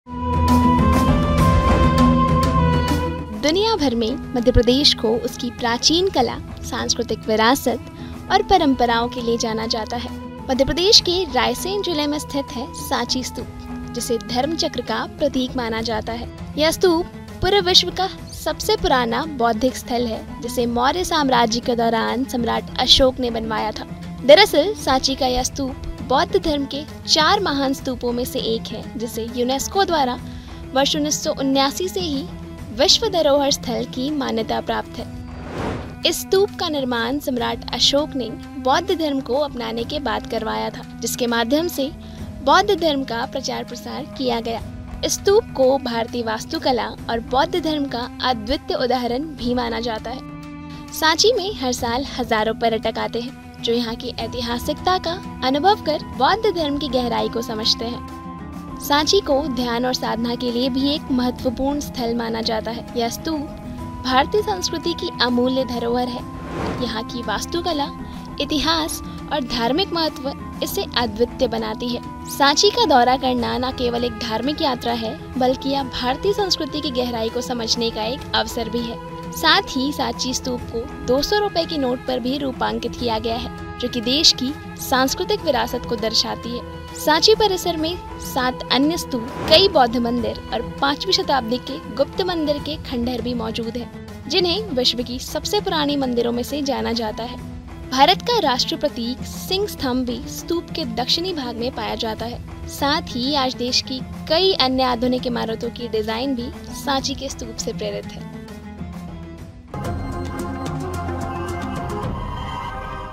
क्यों माना जाता सांची को धर्म चक्र का प्रतीक ? | Sanchi Stupa | MP | MP Toursim
क्यों माना जाता सांची को धर्म चक्र का प्रतीक ?

मध्य प्रदेश के रायसेन जिले में स्थित है सांची का स्तूप जिसे धर्म चक्र का प्रतीक माना जाता है। यह स्तूप पूरे विश्व का सबसे पुराना बौद्धिक स्थल हैं जिसे मौर्य साम्राज्य के दौरान सम्राट अशोक ने बनवाया था। दरअसल, सांची का यह स्तूप बौद्ध धर्म के चार महान स्तूपों में से एक है जिसे यूनेस्को द्वारा वर्ष 1989 से ही विश्व धरोहर स्थल की मान्यता प्राप्त है।

The Sanchi Stupa, located in Raisen district of Madhya Pradesh, is considered a symbol of the Dharma Chakra. This stupa is the oldest Buddhist site in the world, built during the Mauryan Empire by Emperor Ashoka. In fact, the Sanchi Stupa is one of the four great Buddhist stupas, recognized as a UNESCO World Heritage Site since 1989

देखिए BSTV MP-CG टेलीविजन चैनल
Airtel DTH-375